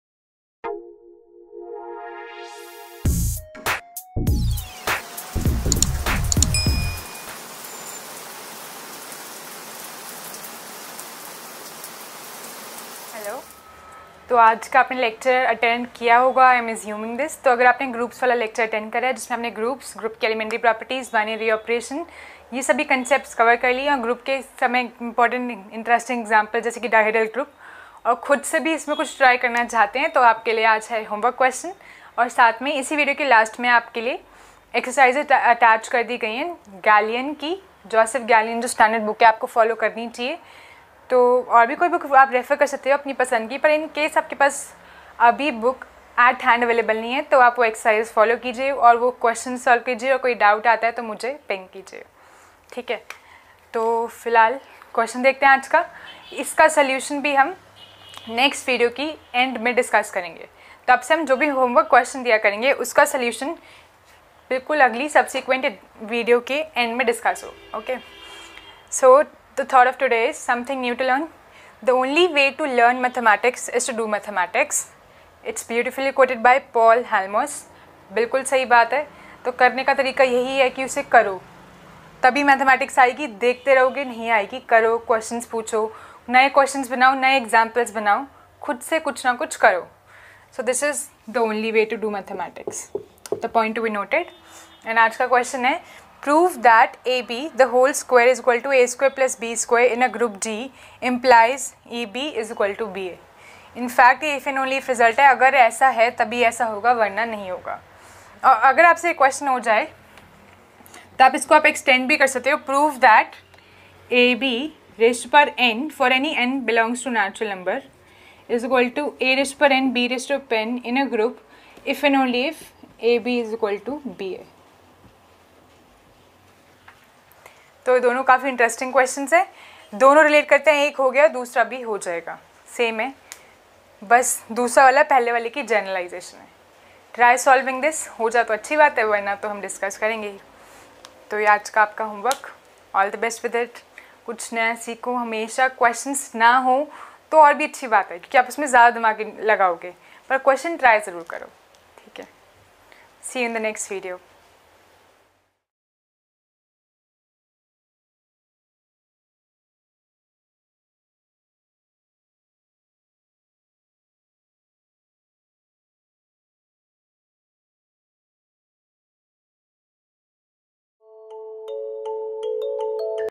Groups Practice Questions + Exercises from Joseph A.Gallian |MSc, DU, ISI, BHU, IIT JAM, BSc(H), CSI
This is a very useful video about the binary operations, groups and related exercise questions, an important way to practice Abstract Algebra especially for students of Mathematics related to DU, TIFR, IITJAM MATHS or those preparing for any masters exam in mathematics or for CSIR NET.

Behind the scenes : (Special thanks)
Special Credit: Family (for being silent till I shoot)

2. Instagram :
- hashtag tripbohemia